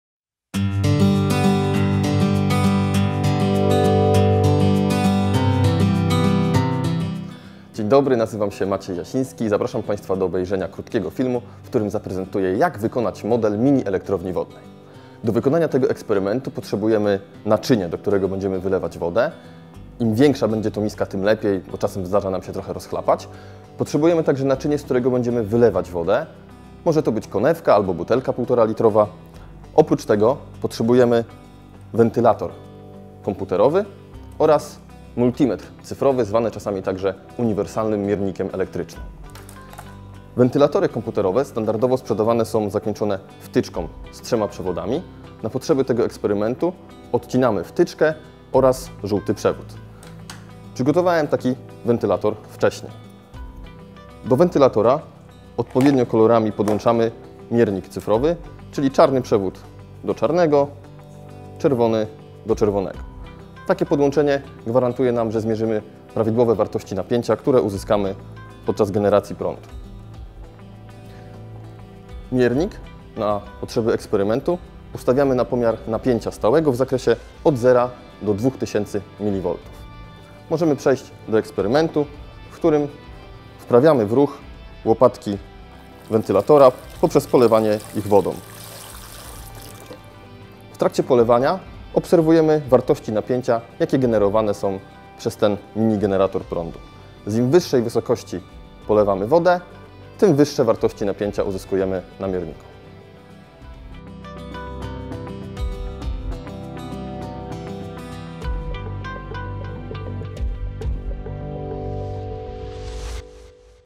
Jak zbudować mini elektrownię wodną? film instruktażowy
Maciej Jasiński pokazuje w jaki sposób można zbudować mini elektrownię wodną.
Film jest elementem zajęć "Czy rzeka może ogrzać miasto?", który jest dostępny na stronie